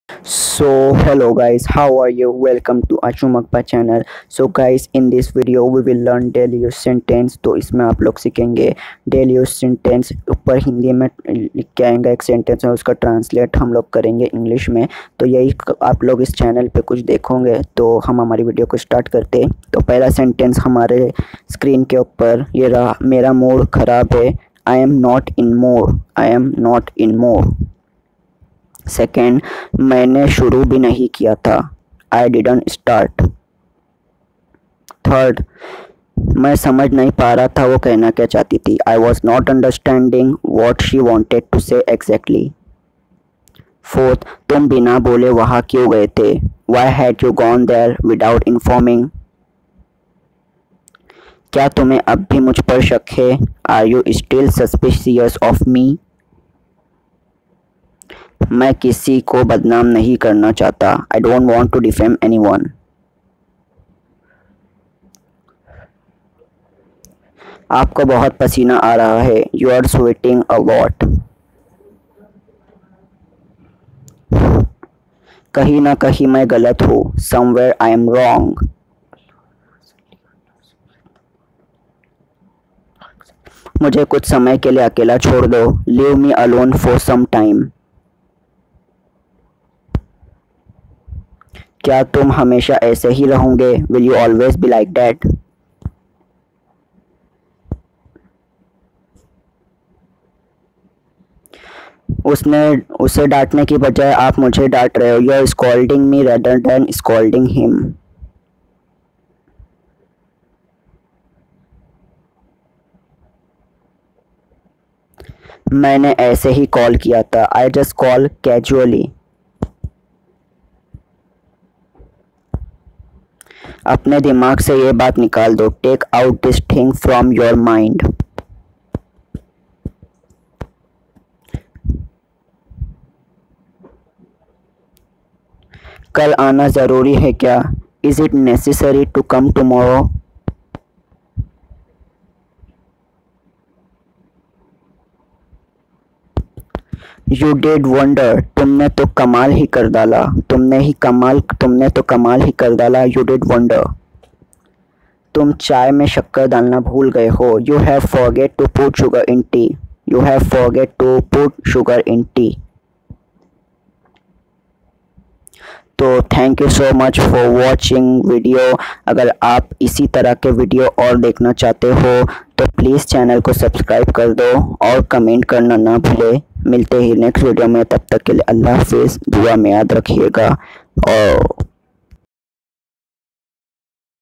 Daily use sentence Part 01/how to learn English/how to speak English/daily hindi to english sentence
Hello dosto

aaj ka hamara video daily use sentence ke taur par banaya gaya h jaha aap kuch hindi ke sentence ko English me kaise bolte h wo sikhege ye hamara pehla video hai to aap channel subscribe kar dijiye Or video ko sabse ke sath share kar dijiye

is channel par daily use sentence hindi se English me kaise bolte hai sikhege